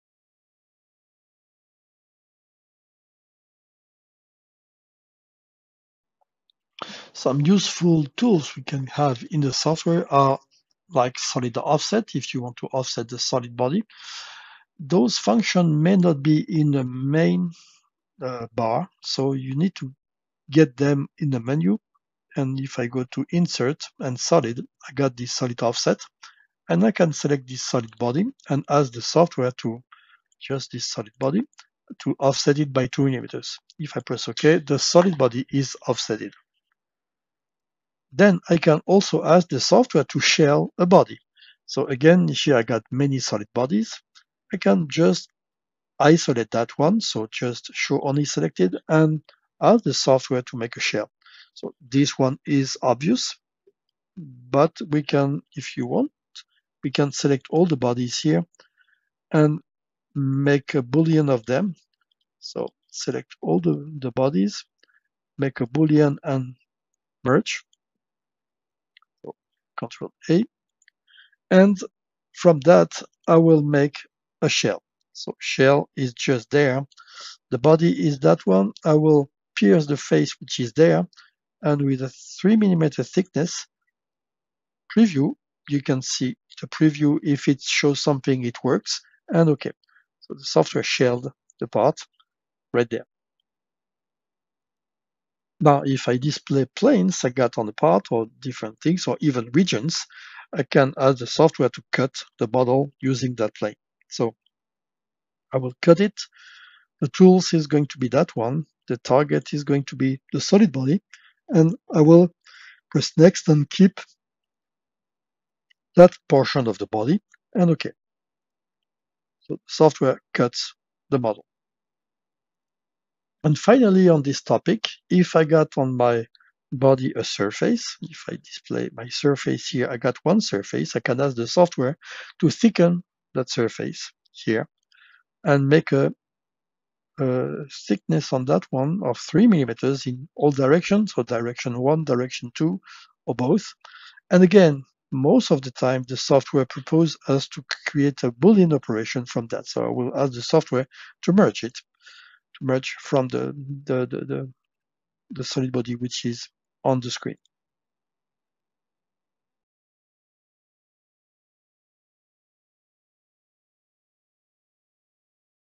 Advanced Solid Tools | Geomagic Design X Tutorial
This tutorial covers how to use the following Advanced Solid Tools in Geomagic Design X:
- Solid Offset
- Shell
- Thicken Surface

These tools are available in Geomagic Design X Plus and Pro.

Speak to sales:
Japan: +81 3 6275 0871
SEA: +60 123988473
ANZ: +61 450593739